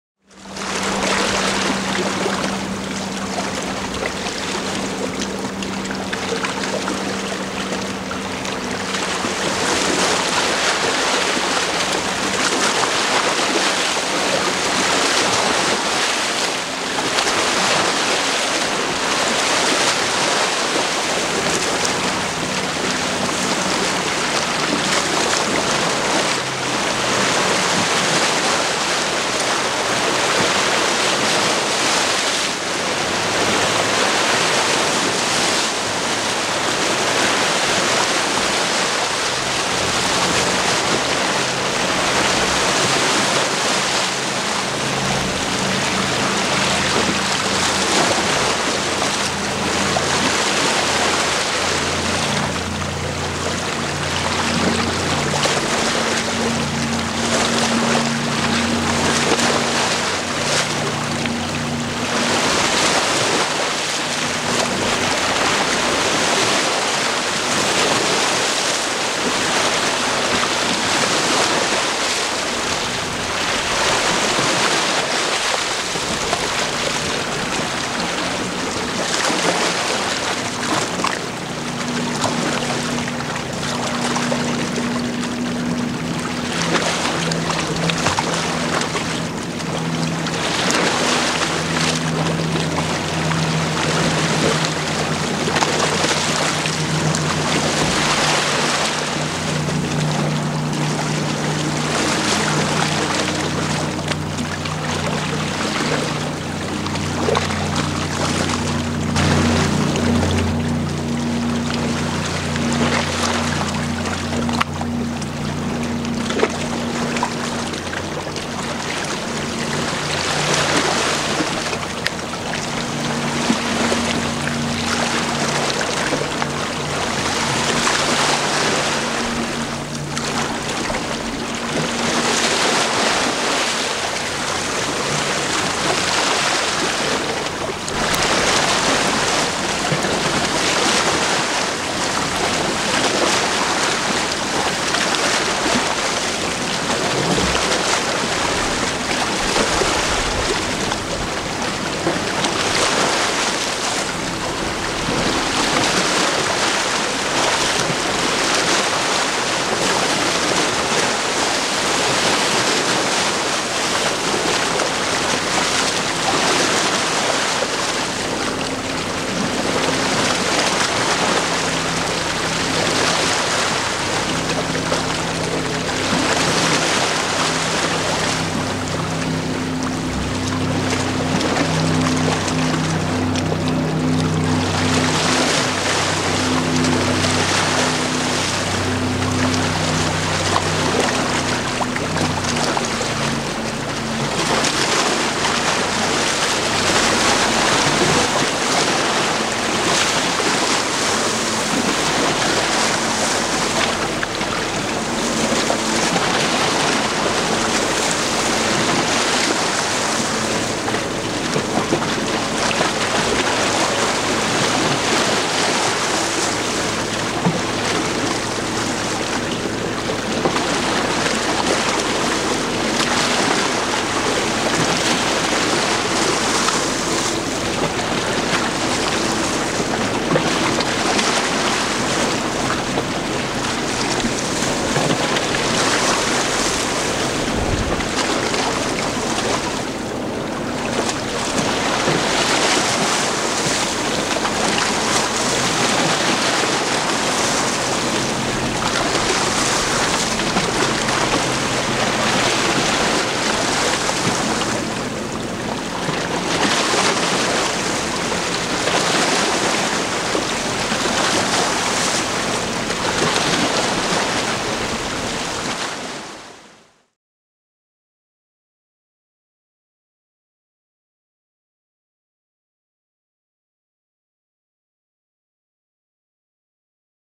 Lyulin (Salty) Sailing on Lake Huron in Port Huron, MI (6-19-2021)
The Lyulin is a Bulk Carrier/Saltwater Ship that is under the flag of Malta.

The Lyulin was built in 2011 and is 186 meters long (610 ft.) and has a carrying capacity of 19,906 tons.

On this trip the ship was headed for Montreal, QC after departing Sault Ste. Marie, ON.